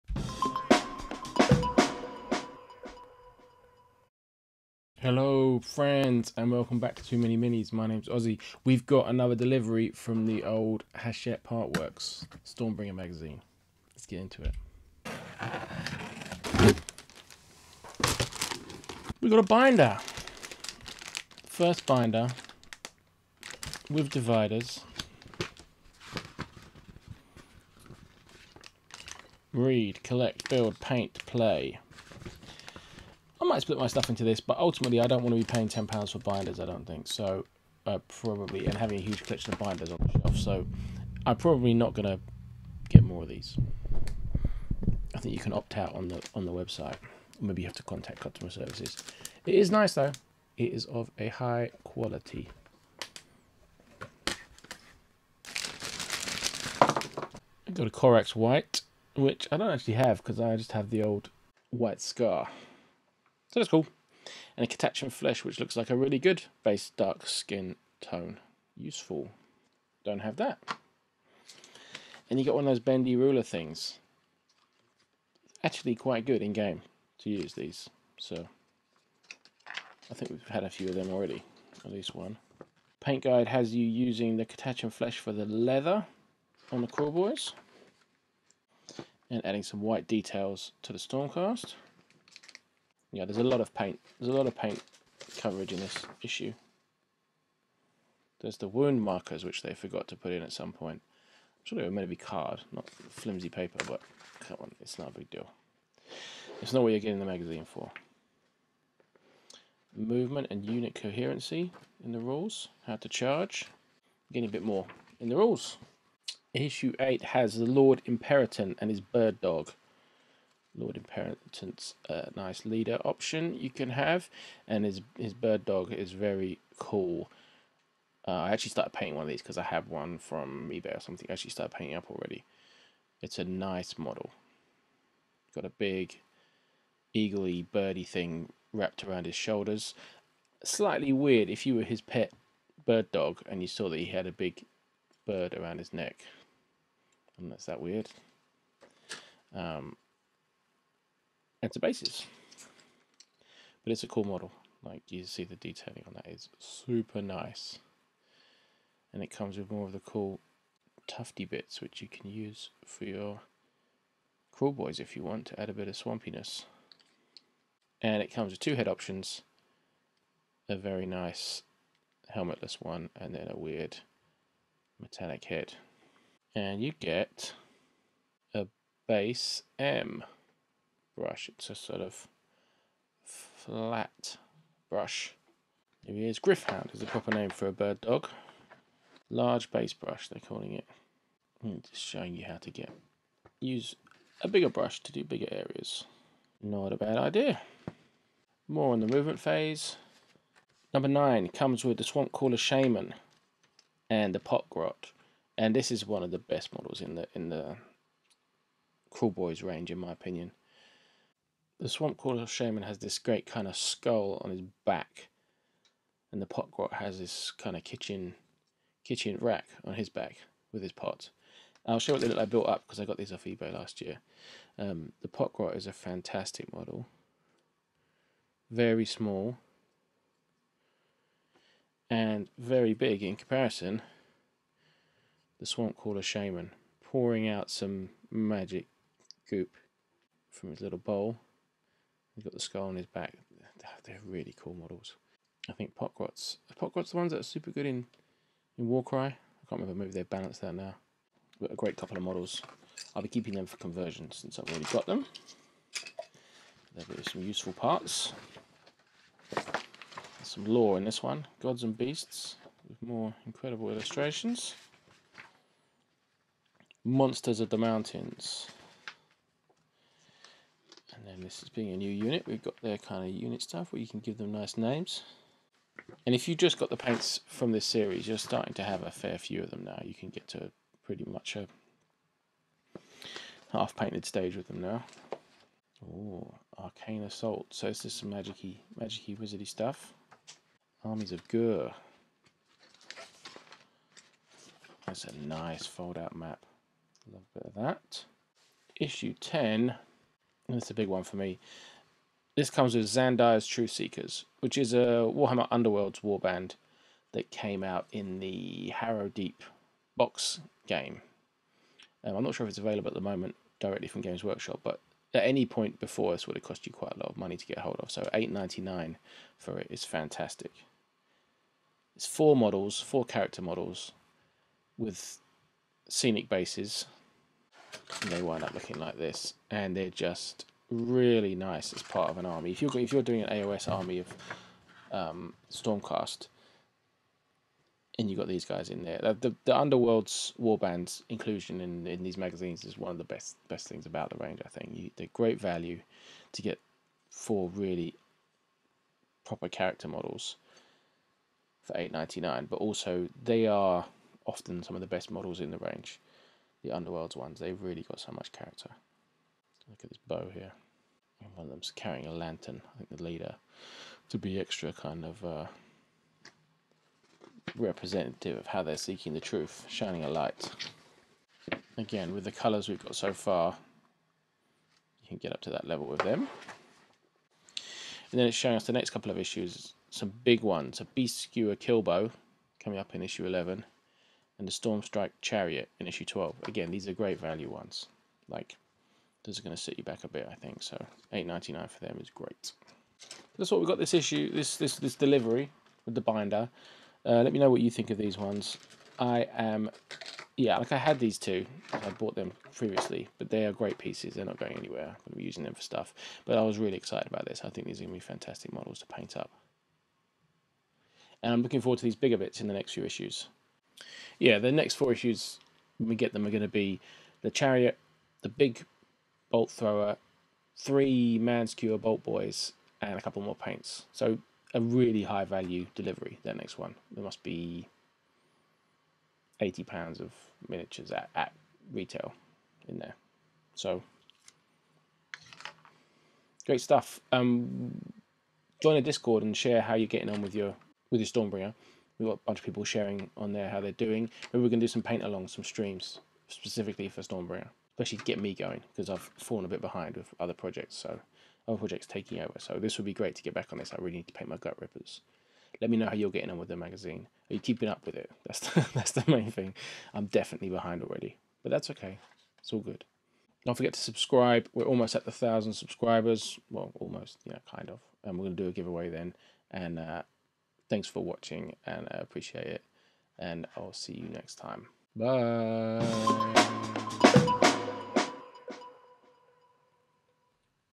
Unboxing Warhammer Stormbringer issues 7, 8, 9 and 10
Ossie explores the third delivery from the Hachette Partworks series.

We'll be doing some Stormbringer paint-along streams on thursday evenings.

If you want to share your Stormbringer painting journey join the Too Many Minis discord server